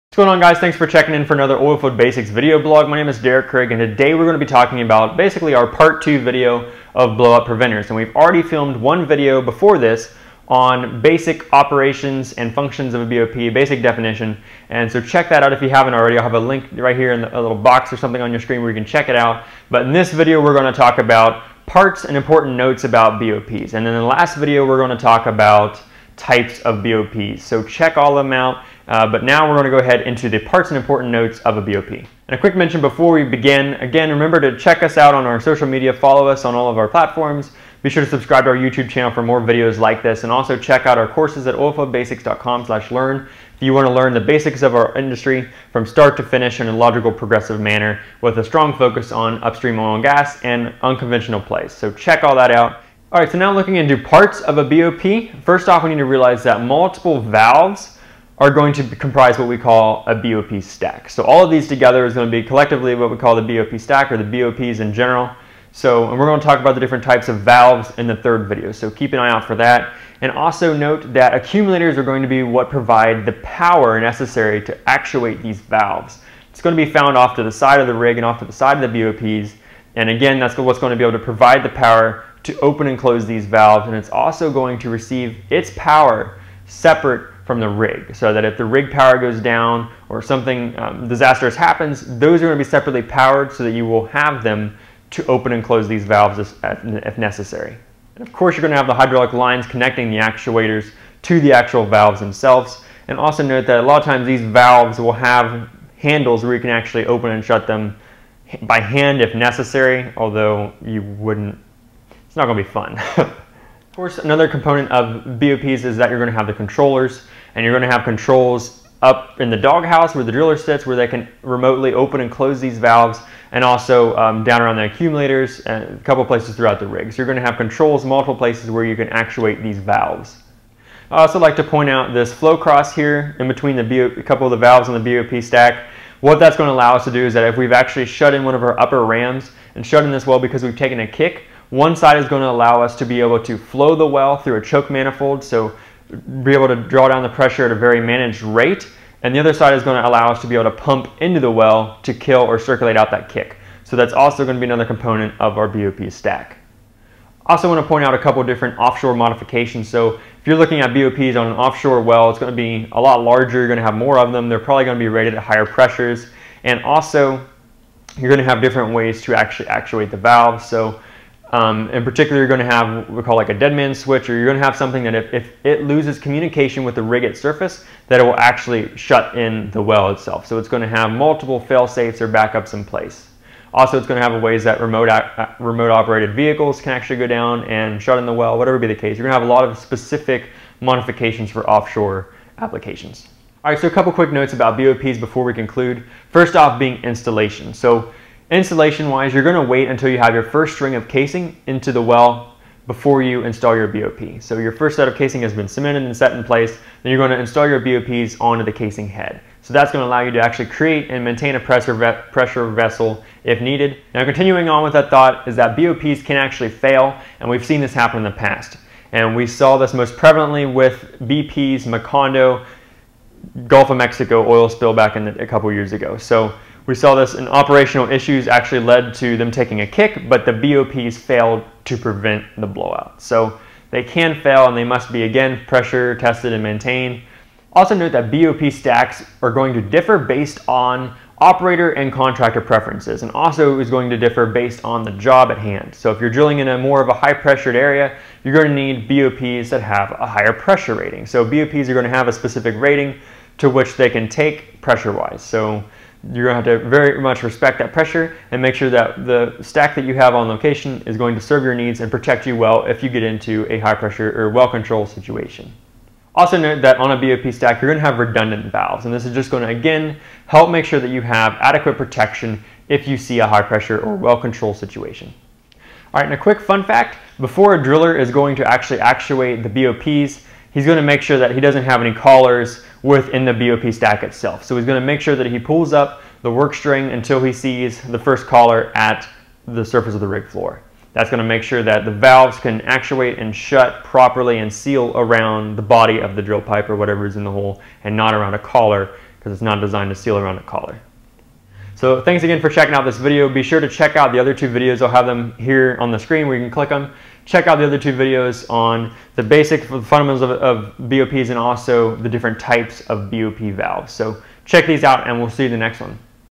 Blowout Preventers - Parts and Notes (2/3)
In this video, Derek reviews the different parts and critical notes of a blowout preventer system.